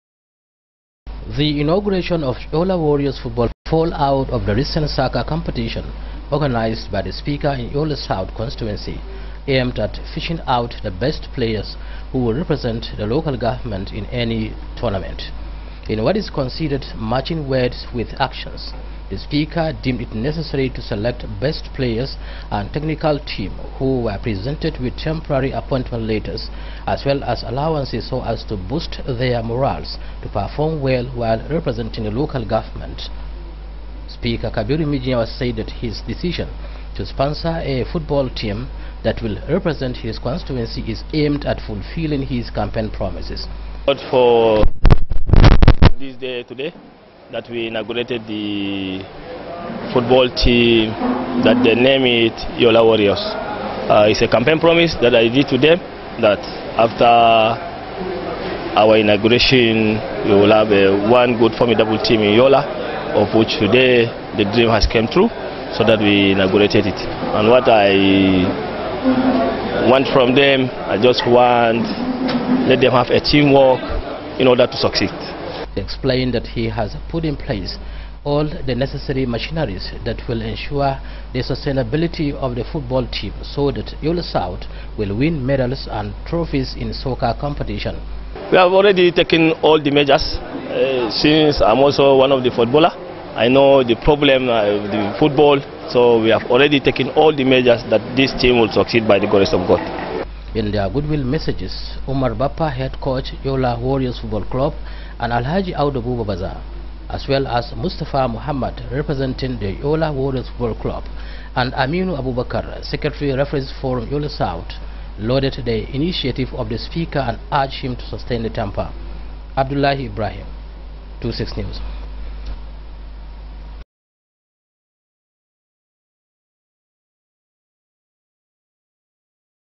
Adamawa Speaker Inaugurates football team and Presents Appointment Letters to team members
Adamawa Speaker Inaugurates football team,Presents appointment letters to team members